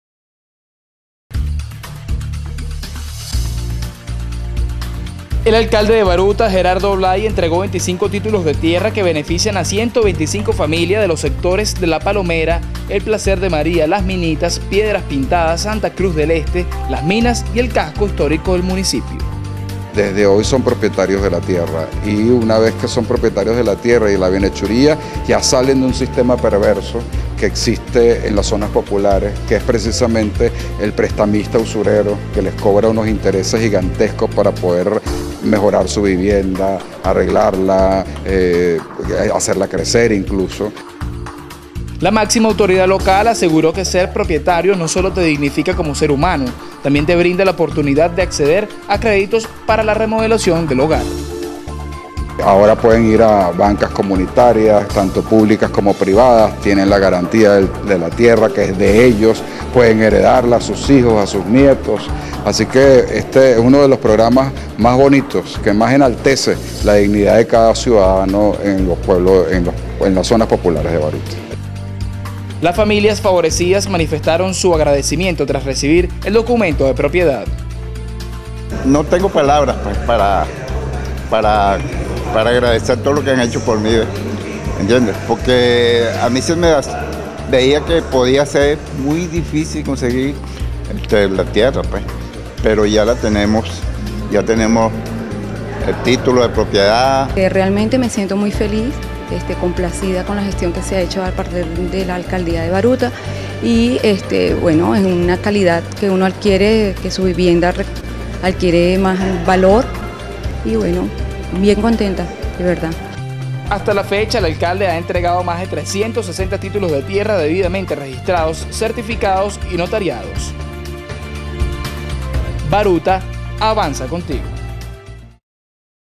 Alcalde Blyde entrega Títulos de Tierras
El alcalde de Baruta Gerardo Blyde entregó  25 títulos de tierra que benefician a 125 familias de los sectores La Palomera, El Placer de María, Las Minitas, Piedras Pintadas, Santa Cruz del Este, Las Minas y el casco histórico del municipio.